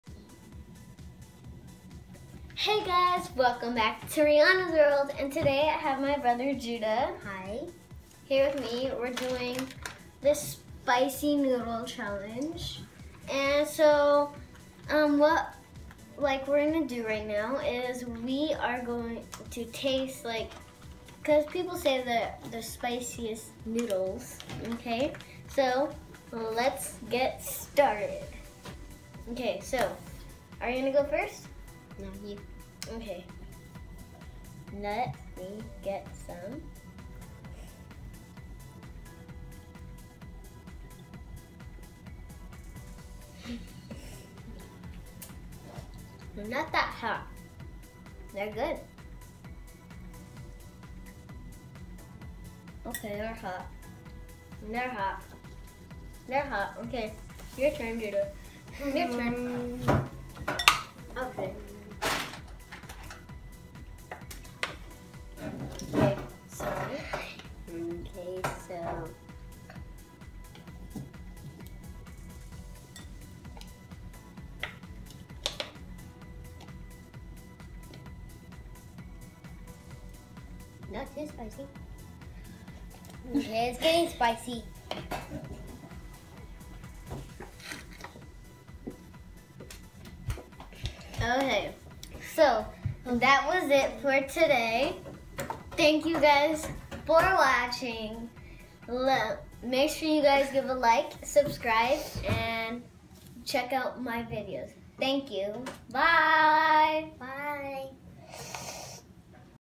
HOT Spicy ramen noodle challenge!!
Rhianna and her brother trying for the first time “HOT & SPICY ramen noodles”

Rhi Rhi - Rhianna